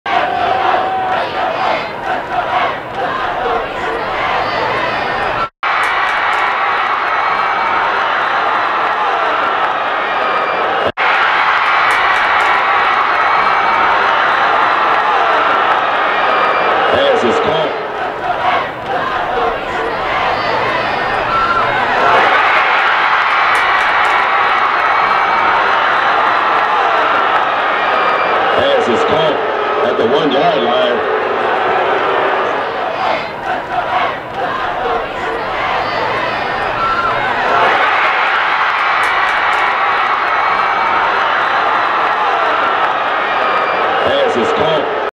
Little Rock Central Touchdown or Not 10-01-30.mp4
Ltittle Rock Central High v/s Russellville High
Questionable call on a touchdown that wasn't.
As the runner steps on the line and then spins on the tackle the ball breaks the plain and should of been a touchdown. What do you think.
video by Mrpopeyevideo